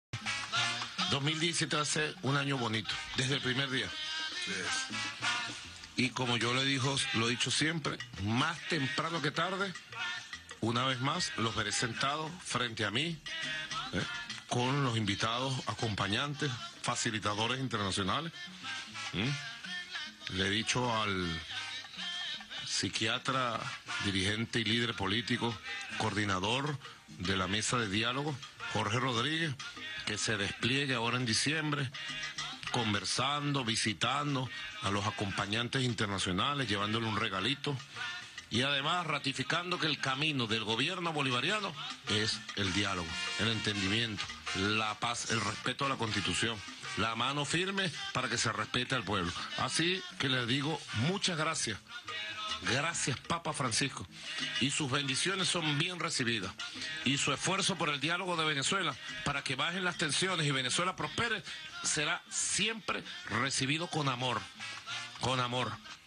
Gobierno ratifica compromiso con el diálogo: “No permitiremos que la MUD llame a la intervención”
El presidente de la República, Nicolás Maduro, ratificó este lunes que el diálogo es la única alternativa que tiene el país para consolidar la paz: “No permitiremos que la MUD llame a la intervención del país”.